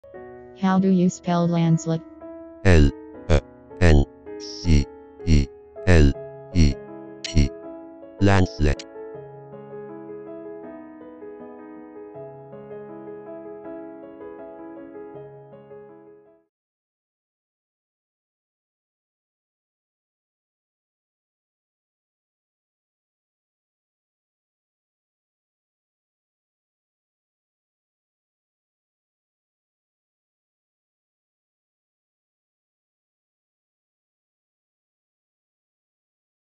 lancelet spelling and pronunciation
Learn how to spell and pronounce lancelet.